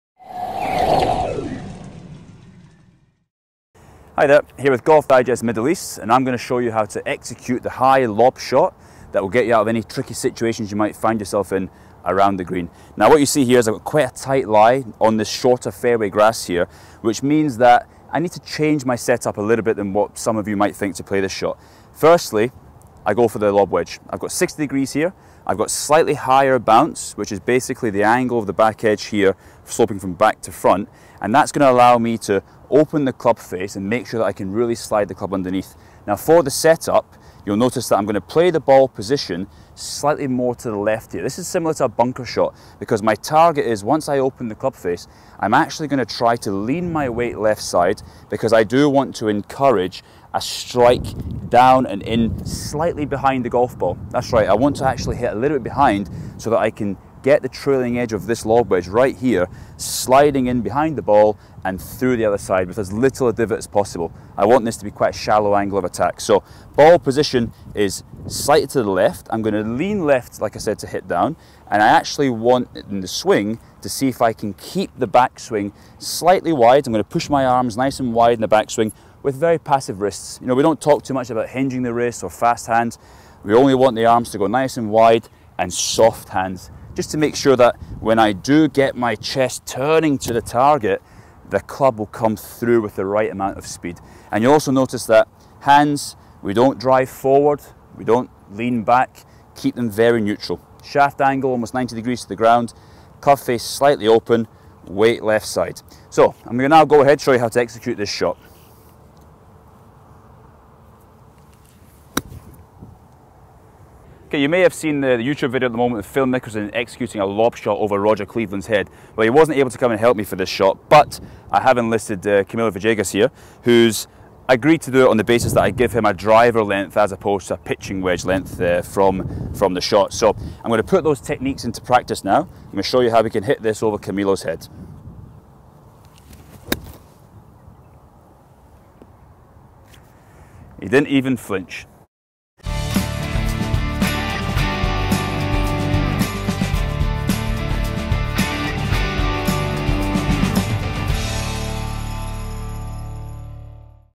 Golf Swing Tuition: How to play the lob shot
Learn how to hit the soft lob shot from a tight fairway lie, and watch Dubai Creek's senior PGA pro George Kasparis recruit a famous volunteer to replicate Phil Mickelson's trademark flop!